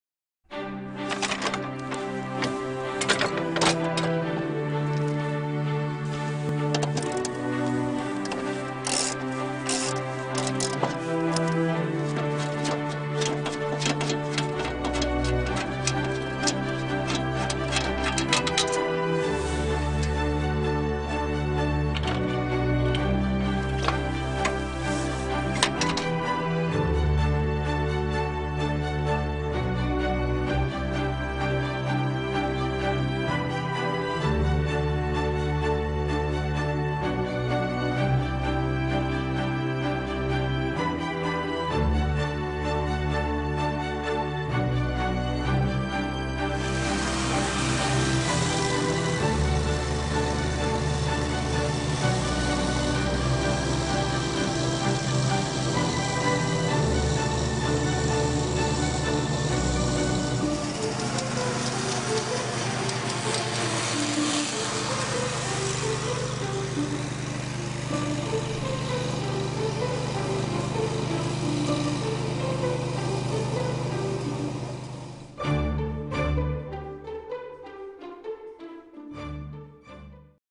Выброс травы назад райдера Husqvarna
Режим выброса травы назад обеспечивает высокую эффективность стрижки переросшей травы, а также редко обрабатываемых газонов. Больше деталей